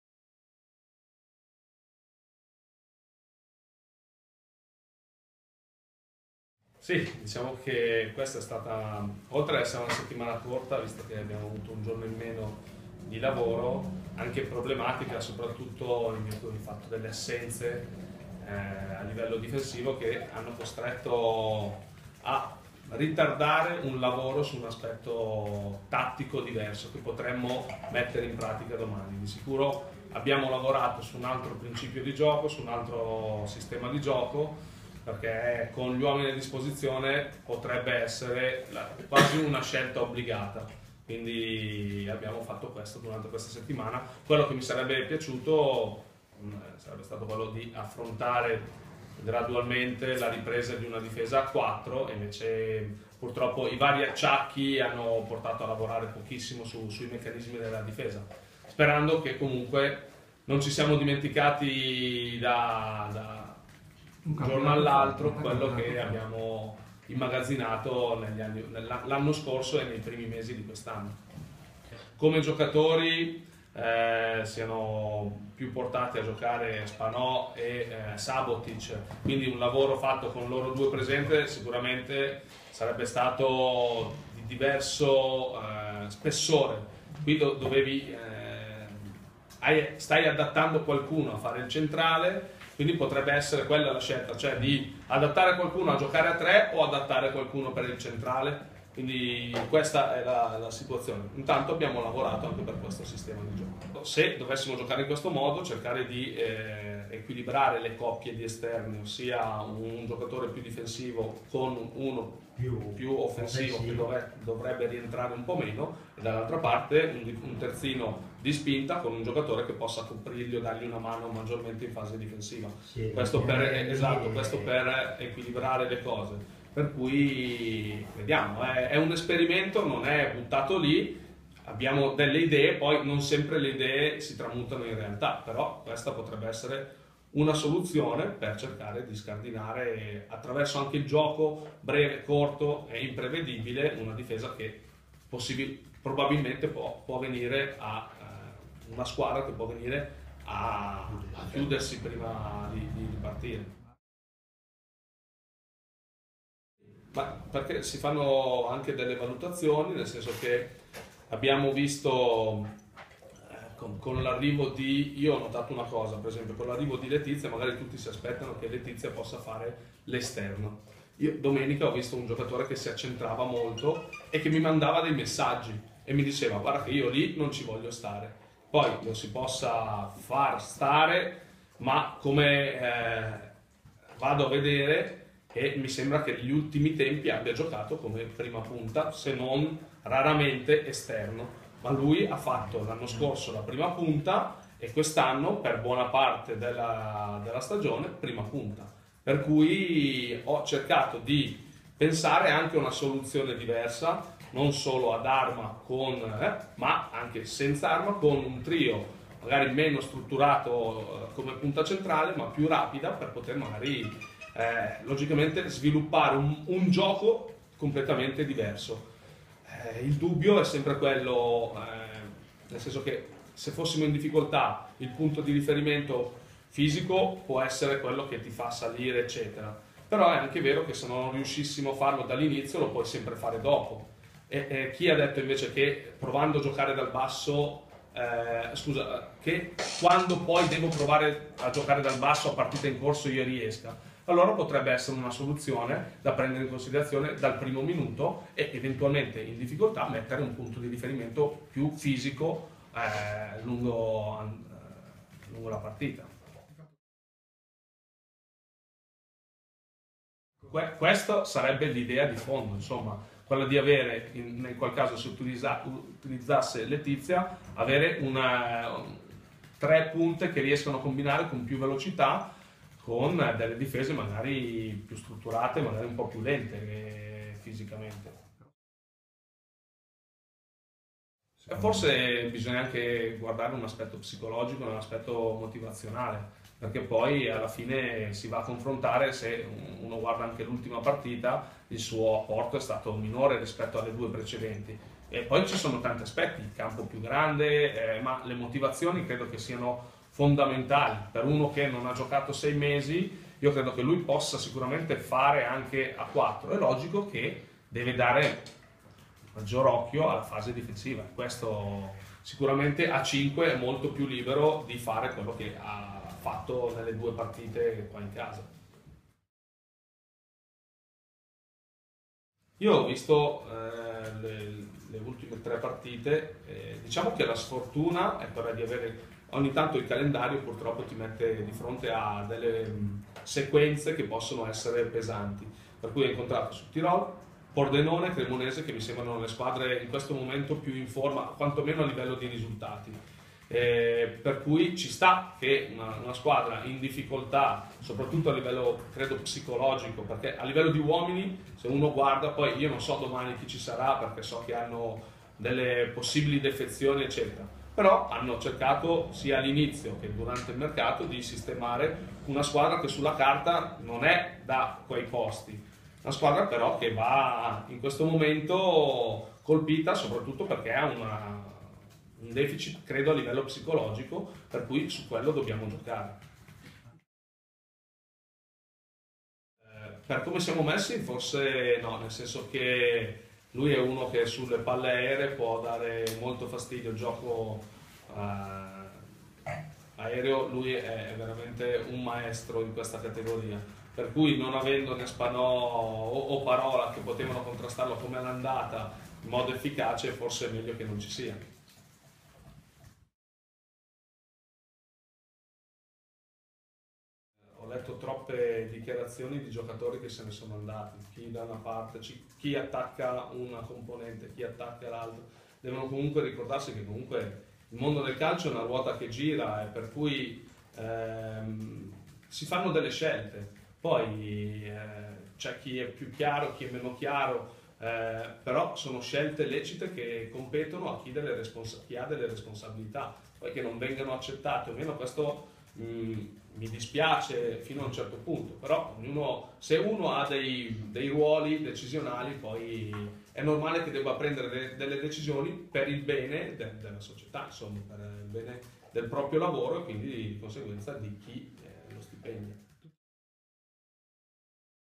Alberto Colombo presenta Reggiana-Mantova
Il tecnico granata presenta il derby della 22ª giornata del campionato di Lega Pro 2015/16 girone A e commenta le parole dell'ex Pesenti contro il dg Ferrara